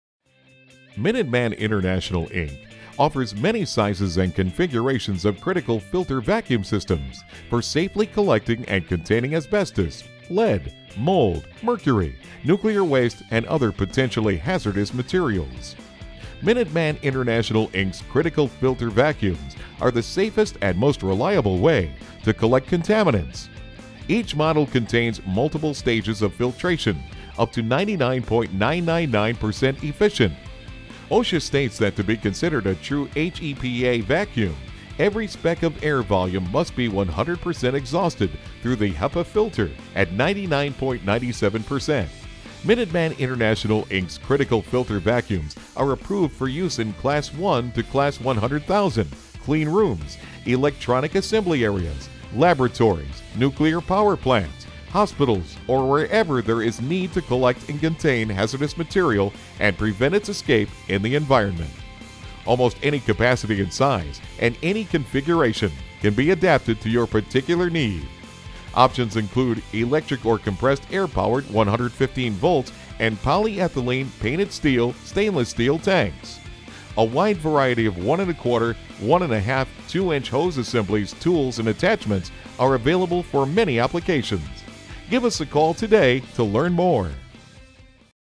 Critical Care Vacuums from Minuteman, Specialty Vacs, HEPA Vacuums
Critical Filter Vacuums by Minuteman International, Inc. offers many sizes and configurations of Critical Filter Vacuum Systems for safely collecting and containing asbestos, lead, mold, mercury, nuclear waste, and other potentially hazardous materials.  Minuteman International, Inc.'s Critical Filter Vacuums are the safest and most reliable way to collect contaminants.  Each model contains multiple stages of filtration up to 99.999% efficient at 0.12 microns.  Filters are tested in accordance with the highest of IEST-RP-CC007 standards. OSHA states that to be considered a true H.E.P.A vacuum, every spec of air volume, must be 100% exhausted through the Hepa filter at 99.97% and 0.3 microns or greater.

Minuteman International, Inc.'s Critical Filter Vacuums are approved for use in Class 1 to Class 100,000 (ISO3) clean rooms, electronic assembly areas, laboratories, nuclear power plants, hospitals, or wherever there is a need to collect and contain hazardous material and prevent its escape in the environment.  Almost any capacity and size -- from 4 gallons up to 55 gallons -- and any configuration -- wet, dry or a combination wet/dry -- can be adapted to your particular need.  Options include: electric or compressed air powered, 115 volts, and polyethylene/painted steel/stainless steel tanks.  A wide variety of 1.25"/1.50"/2.00" hose assemblies, tools and attachments are available for many applications. Give us a call today to learn more.